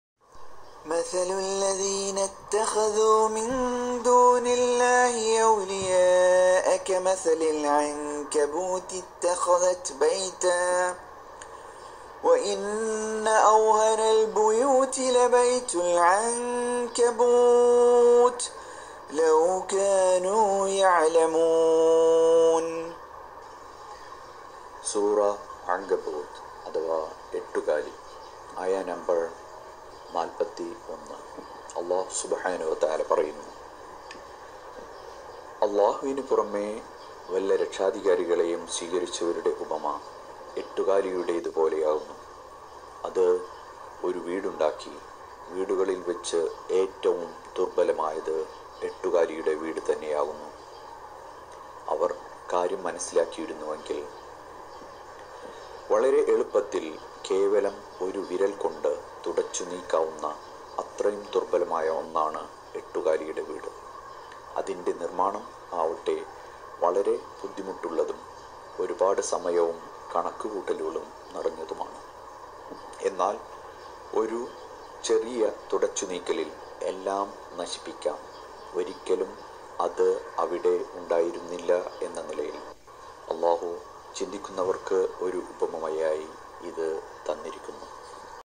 Sura Ankaboot, aya#41 explanation in Malayalam by Muhammed Kamal.
സൂറ അങ്കബൂത് അധവാ എട്ടുകാലി, അയ 41. അല്ലാഹു അല്ലാതെ മറ്റാരെങ്കിലും ആശ്രയിക്കുന്നവർക്കു ഒരു ഉദാഹരണം..a Malayalam talk by Muhammed Kamal..👆